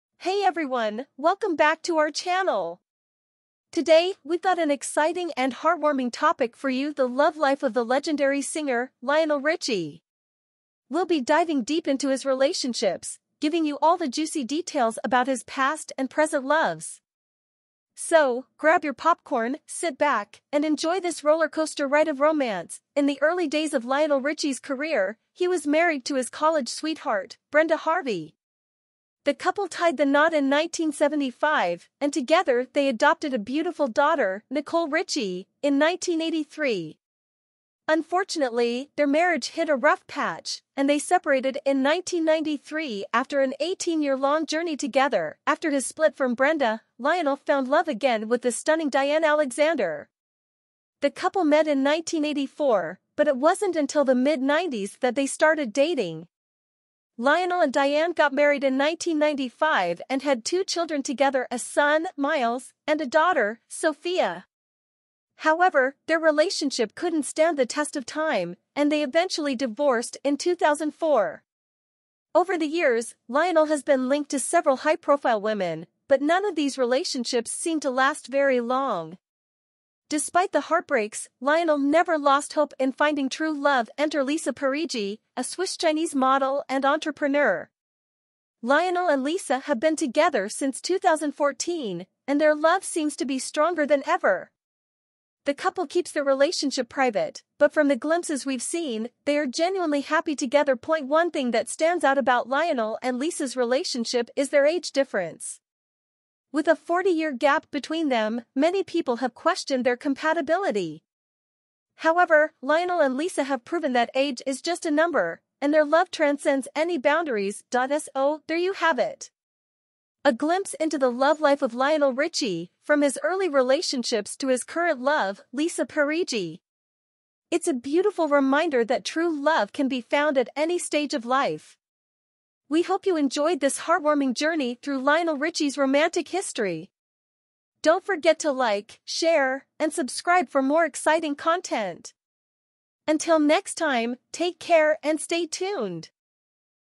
Lionel Richie's Unbelievable ♥️ Love story with Brenda Harvey 🎉
Get an exclusive look into Lionel Richie's love life, from his early romances to his current relationship with Lisa Parigi. Discover the ups, the downs, and the heartwarming moments in this captivating story of love that knows no bounds. Don't miss out on this viral sensation!

Regenerate response